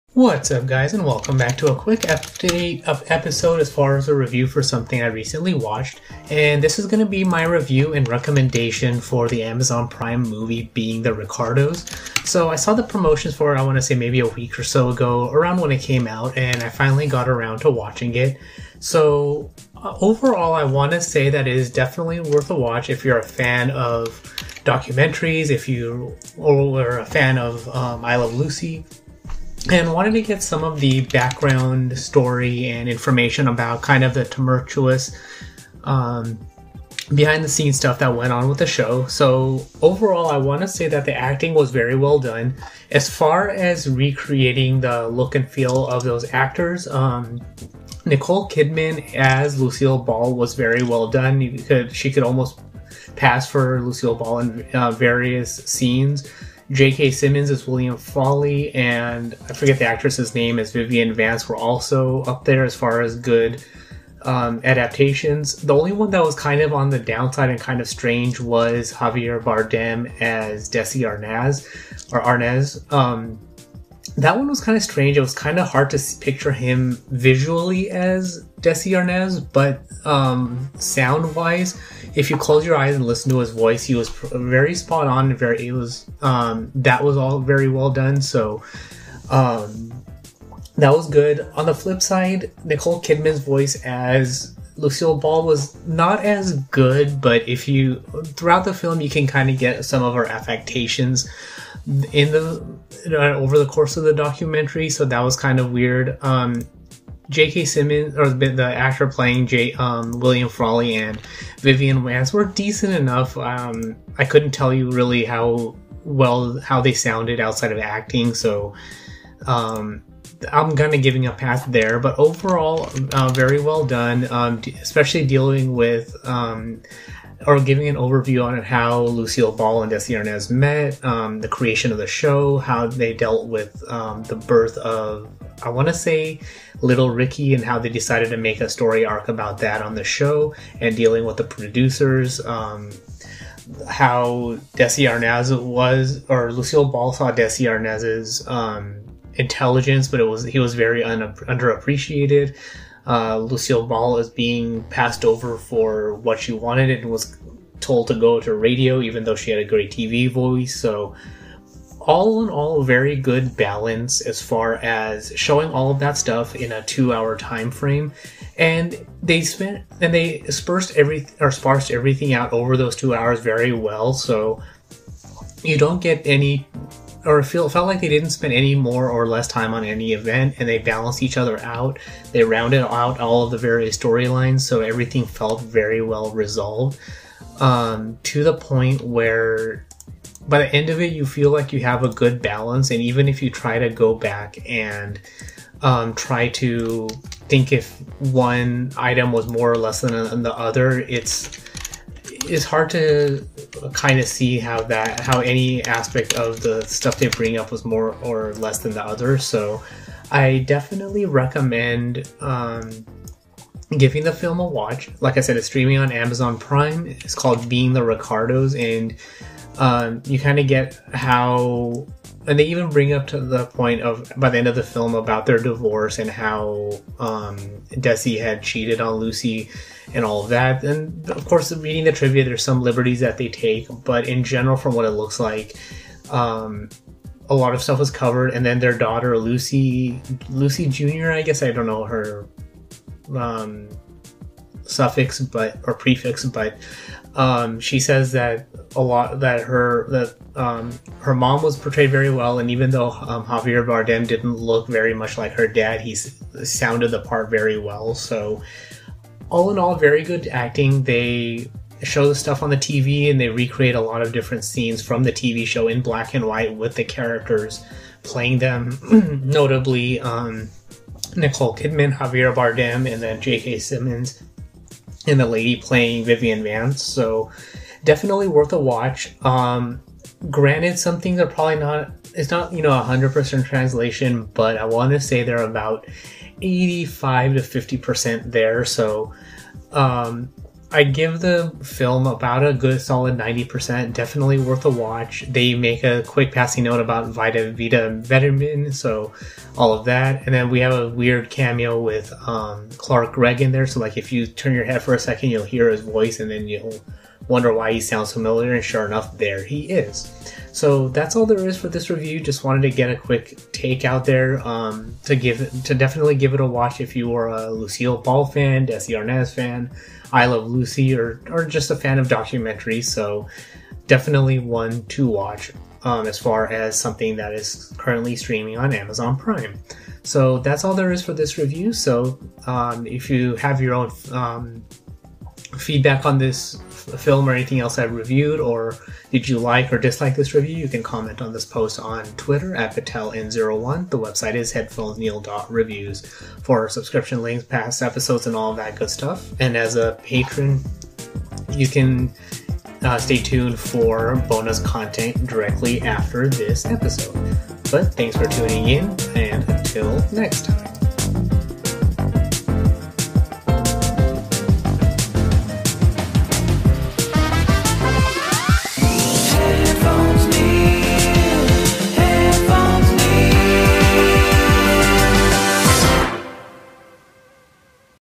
Being the Ricardos - Podcast
Follows Lucy and Desi as they face a crisis that could end their careers and another that could end their marriage.